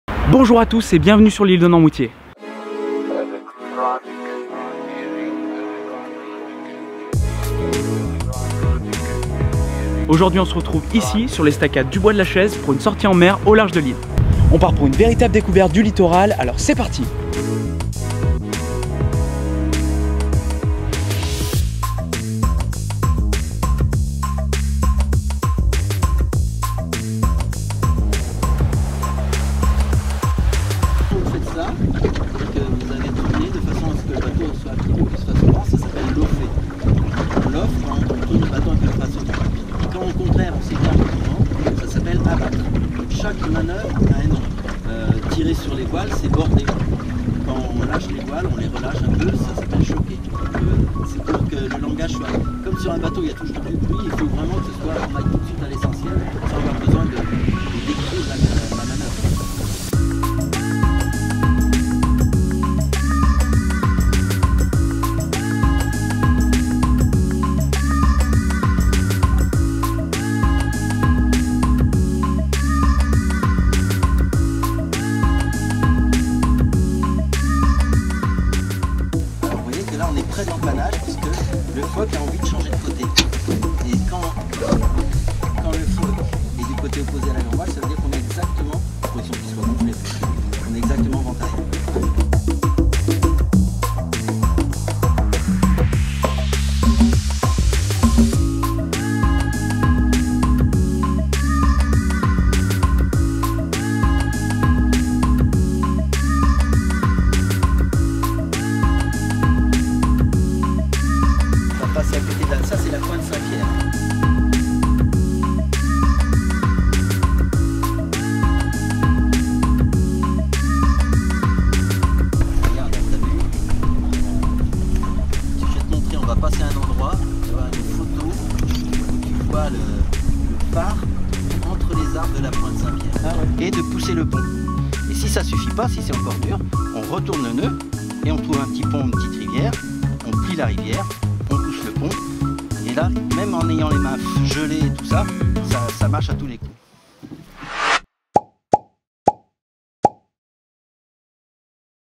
On a testé pour vous... une sortie en mer avec Noirmoutier Littoral !
Embarquez avec nous sur le Queequeg de Noirmoutier Littoral en cliquant ici ⬇️
Rejoignez-nous !